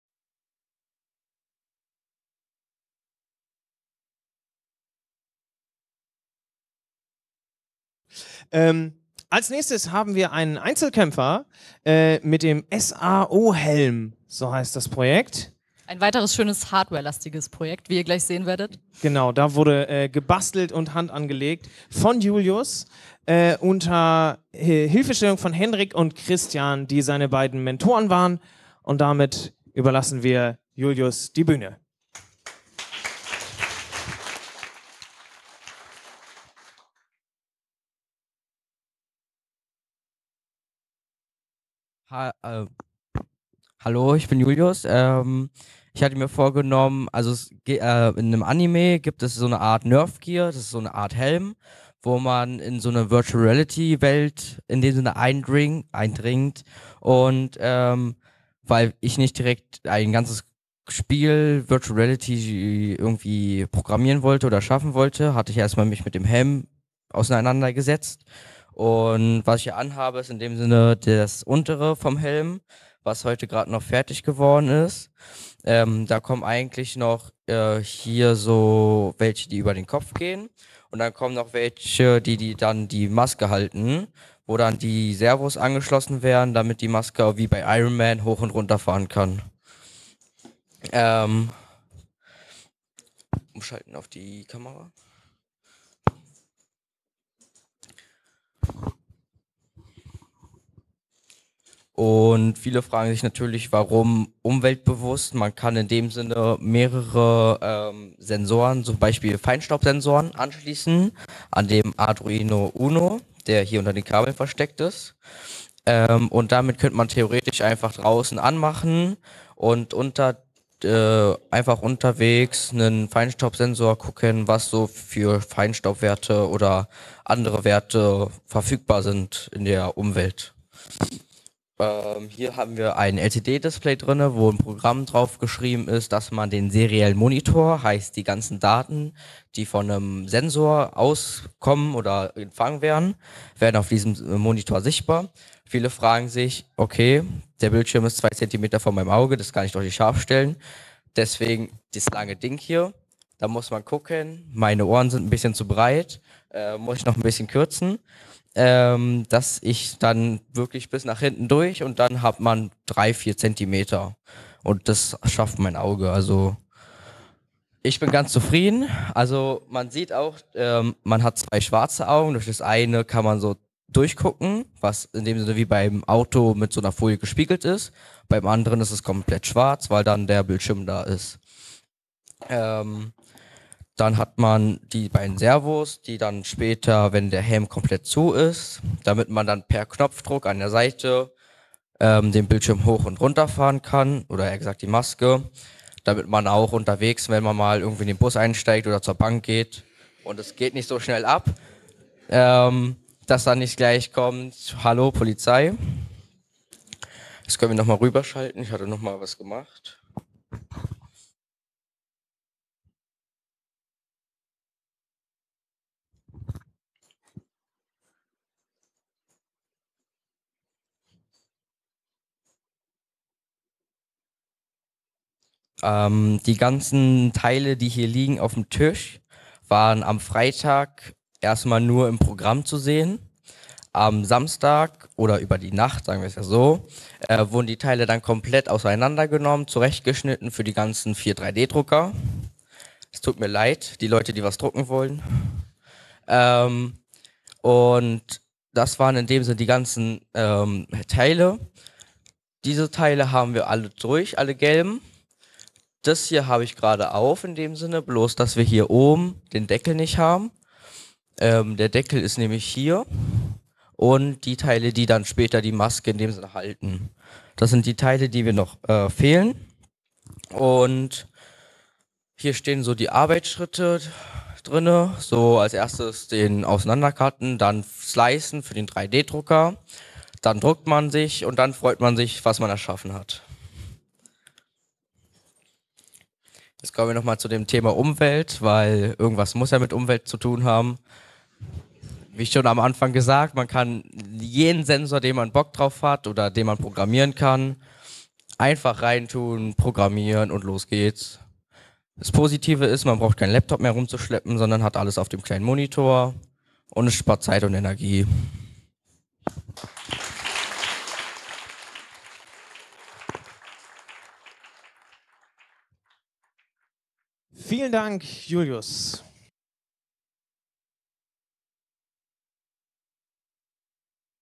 S.A.O. Helm | Jugend hackt Köln 2019
Was Iron Man kann, kann Julius schon lange! Dazu hat er die 3D Drucke einem Belastungstest unterzogen. Mit einem Display im Helm können Umweltdaten live angezeigt werden. Von Julius.